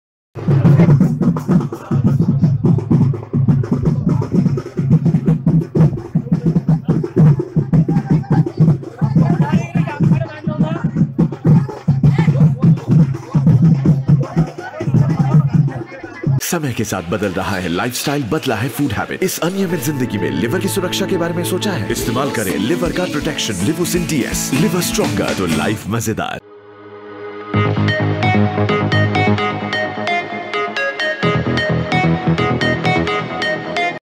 ବିଧାୟକଙ୍କୁ ମାଡ଼ି ବସିଲା କାଳେସୀ | Odisha Reporter
ବିଜେଡି ବିଧାୟକ ରୋହିତ ପୂଜାରୀଙ୍କୁ ଦଣ୍ଡଯାତ୍ରା ବେଳେ କାଳେସୀ ମାଡିବାର ଦୃଶ୍ୟ। ଦେଖନ୍ତୁ କିପରି ବିଧାୟକଙ୍କୁ ଧରିଛନ୍ତି କାଳେସୀ।